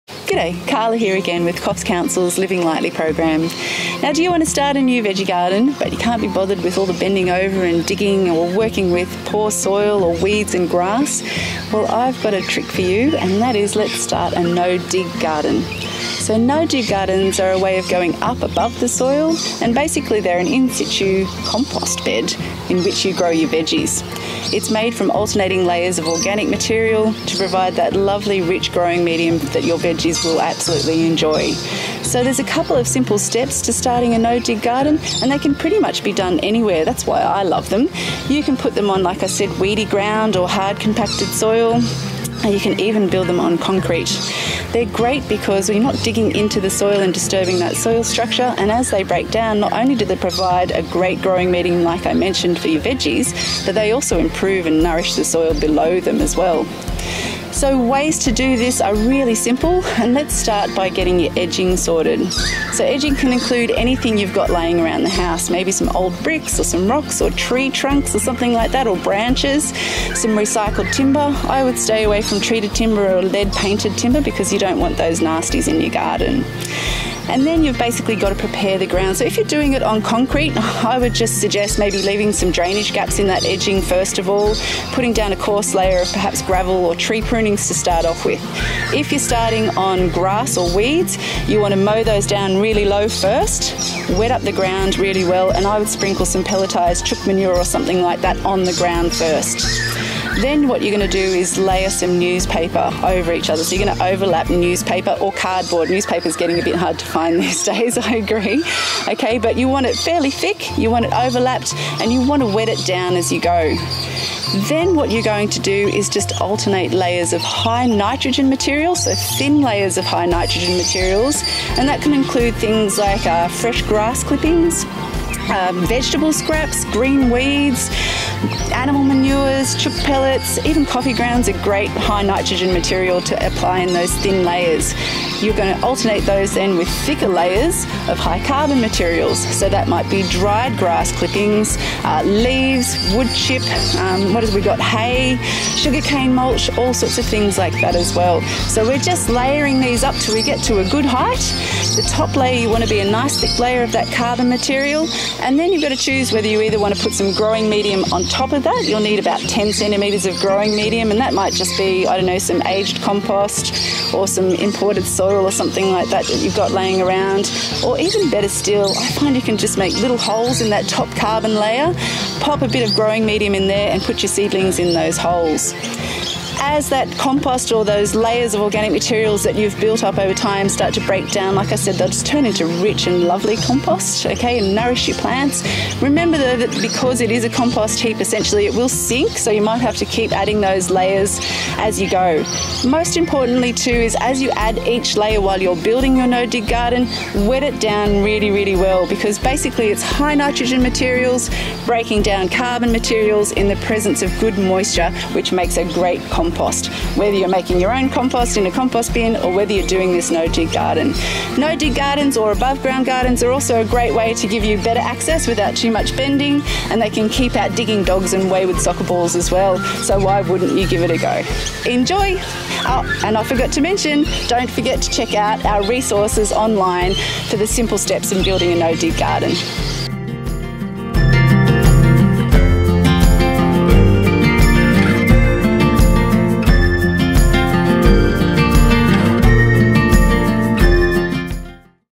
How to start a no-dig veggie garden!
Karla explores the benefits of no-dig veggie gardens and explains how you can create one at home!